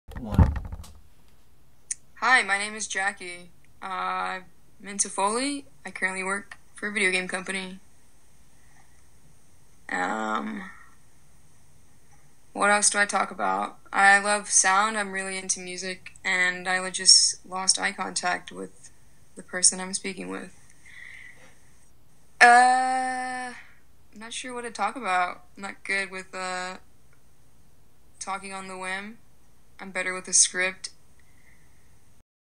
Learning Sound -- I'm becoming a Foley Sound Artist
I'm Jackie.  This is the first day of my journey to become a professional foley artist.  I hope I can make it!